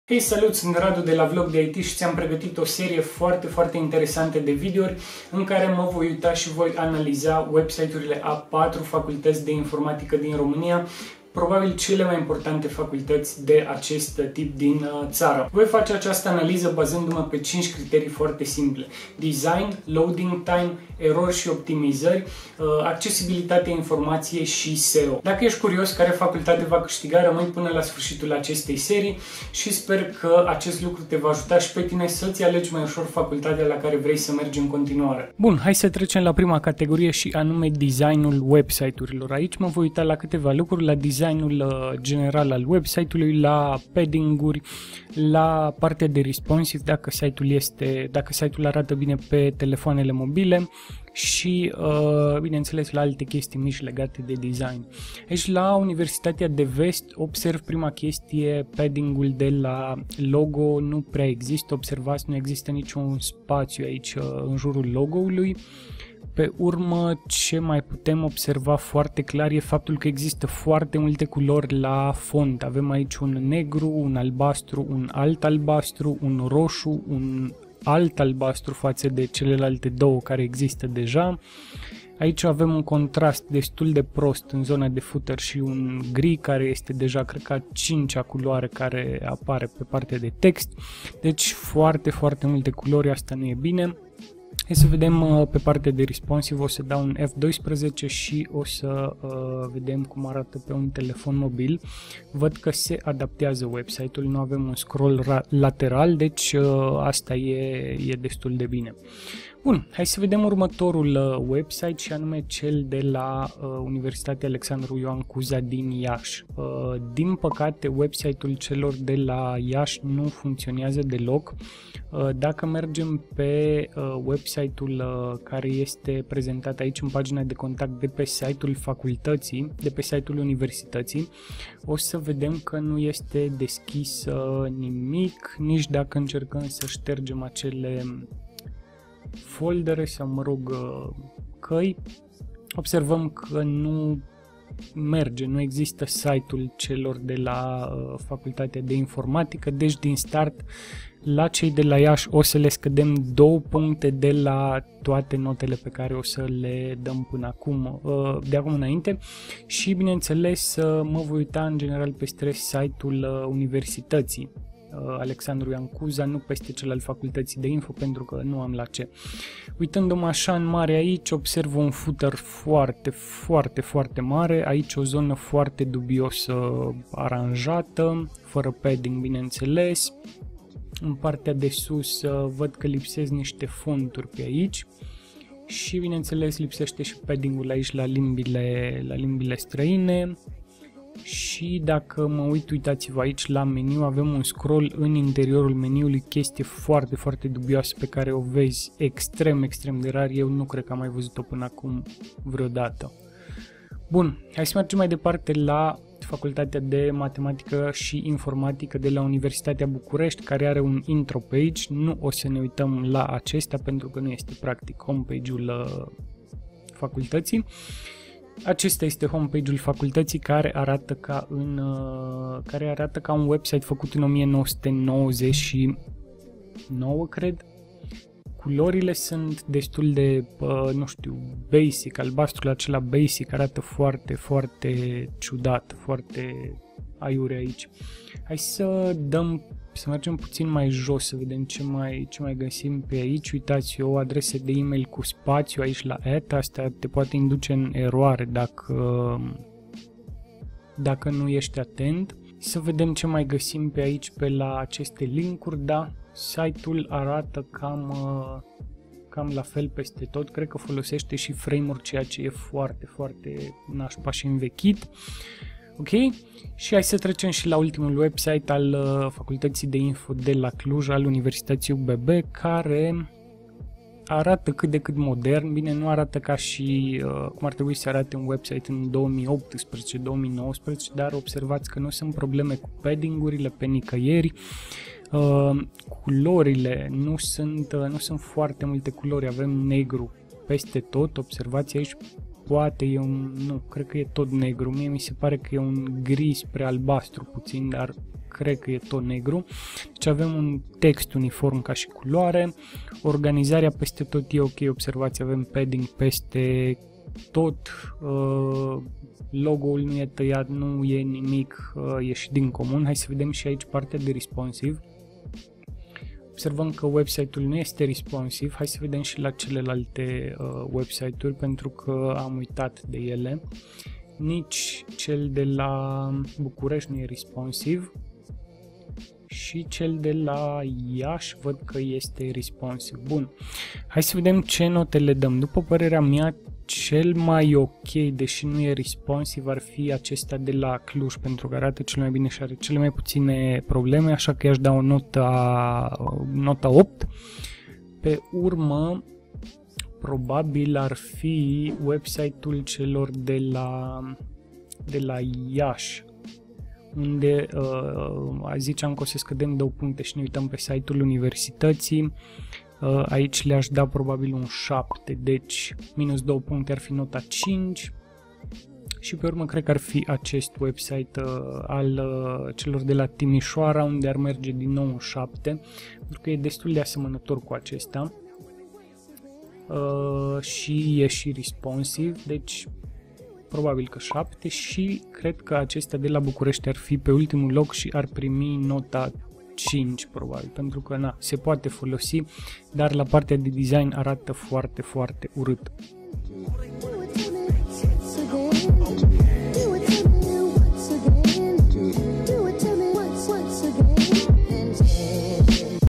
Analizez siturile facultatilor de informatica (1/6)
In episodul de azi ma uit peste partea de Design a celor 4 web-site-uri ale facultatilor de informatica din Iasi, Cluj, Timisoara si Bucuresti. Hai sa vedem ce note primesc. ↓↓↓ Mai multe informatii ↓↓↓